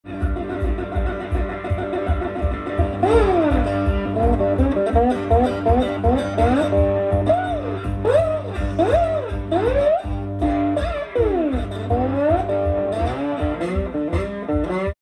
Rocking Out On A Guitar Made Out Of A Shovel! Only 3 Strings Can Make This Much Sound!
Rocking out at The Venice Gazebo In Venice Florida On A Friday Night playing my 3 String Shovel Guitar.
Steve Arvey always enjoys the sound of homemade instruments. Steve gets a real raw slide sound out of this shovel guitar made by Doug McDowell of Devil’s Broomstick Guitars. Arvey is using his patented Killer Strings on this instrument which gauges are 42 32 22 and is tuned GDG